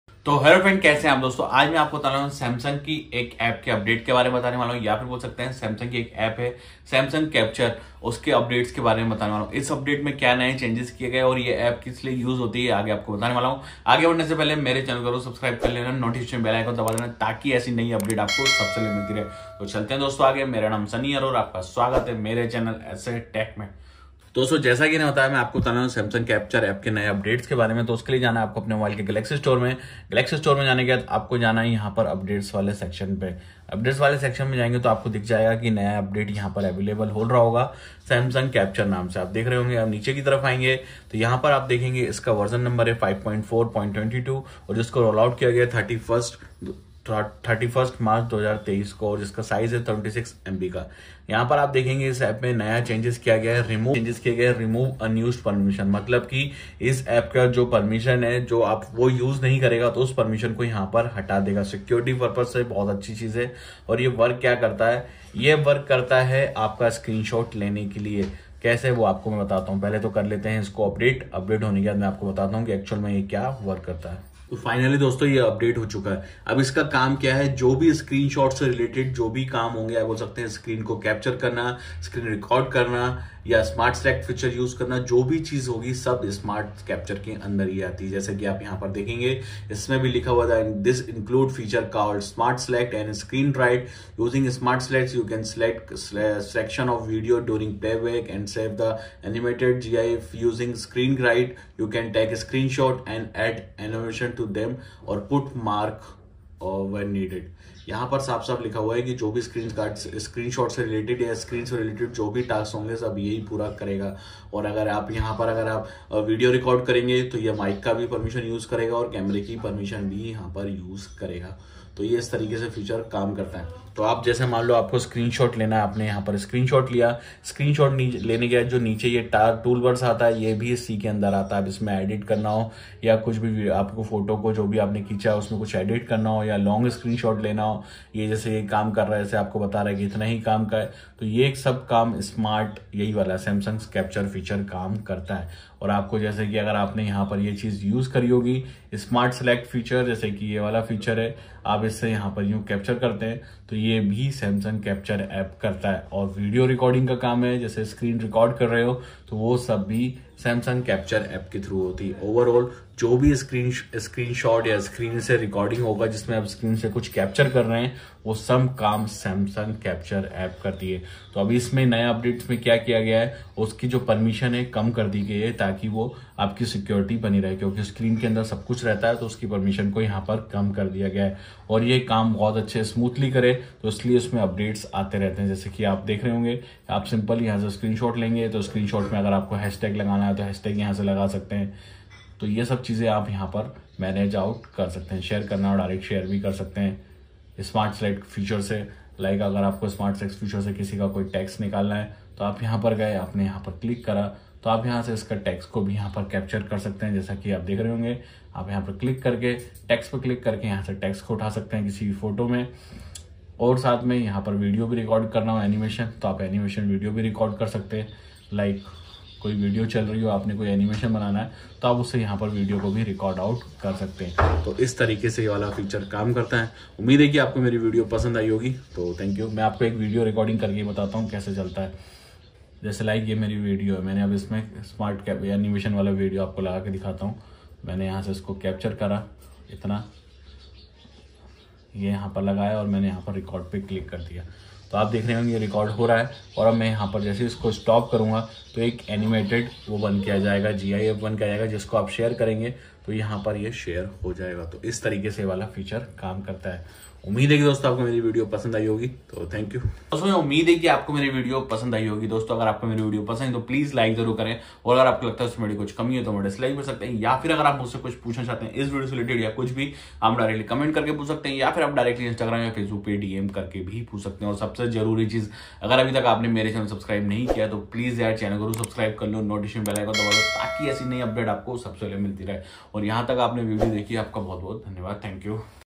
Samsung Capture App New Update 2023, What is Samsung Capture App ?
in this video i describe about samsung capture app new update and features march 2023